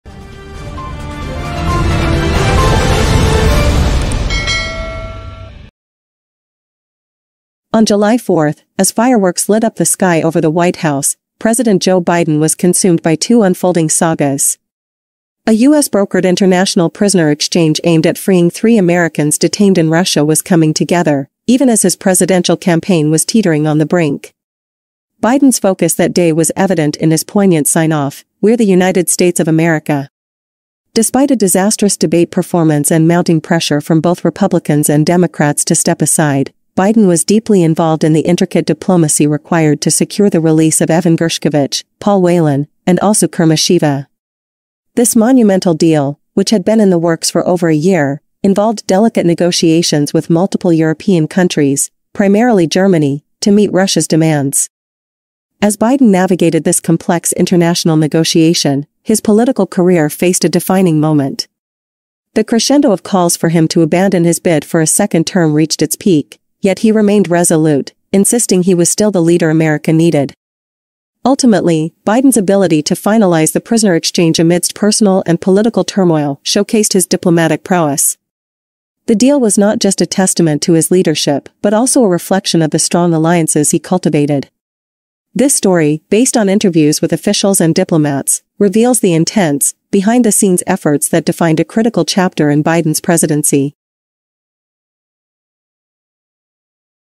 Biden’s Secret Deal: The Shocking Truth Behind His July 4 Fireworks
As fireworks exploded over the White House on July 4, President Joe Biden's mind was on a daring mission and a crumbling campaign. Behind the scenes, a high-stakes U.S.-brokered prisoner exchange aimed at freeing three Americans from Russian custody was unfolding. Biden’s poignant July 4 sign-off, “We’re the United States of America,” masked the intense diplomatic efforts happening in secret.

Despite a humiliating debate performance and mounting pressure from both parties to step aside, Biden was laser-focused on the intricate negotiations. This complex deal involved securing Germany's cooperation to meet Russia's demands, demonstrating Biden’s diplomatic acumen even as his political future hung by a thread.

In mid-July, Biden faced a crescendo of calls for him to abandon his re-election bid. Yet, amidst the political storm, he worked tirelessly to finalize the prisoner exchange, showcasing his leadership and resilience. This story, built on insights from key officials and diplomats, uncovers the dramatic behind-the-scenes efforts that defined a pivotal moment in Biden’s presidency.

Category:
Politics